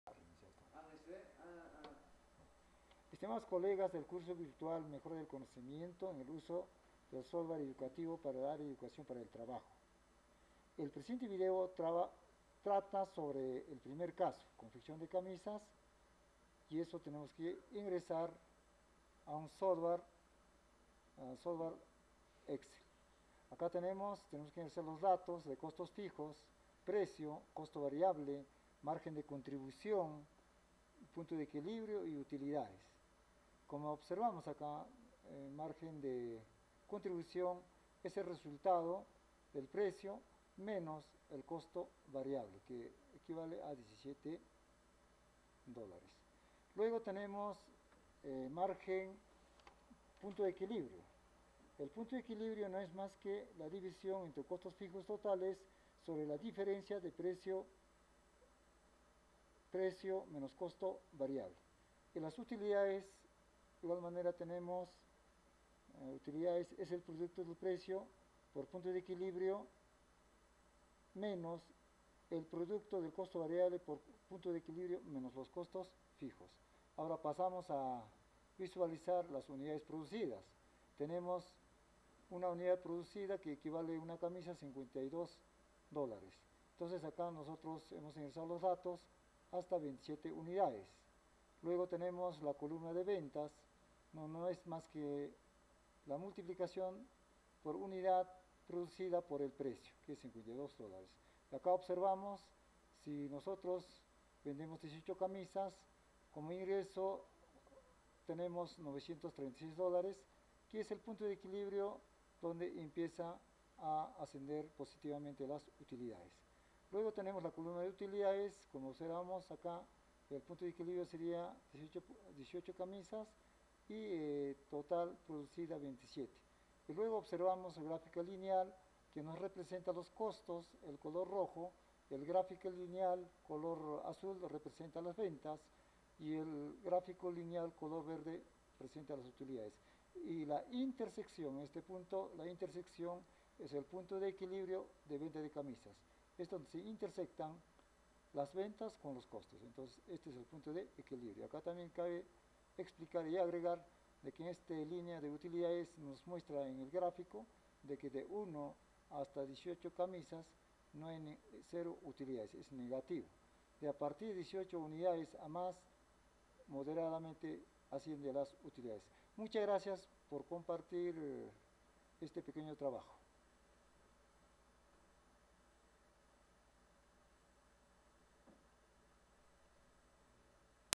Trabajo Adolfo caso 1
Trabajo de punto de equilibrio de la venta de camisas, resuelto en excel con el aplicativo CamStudio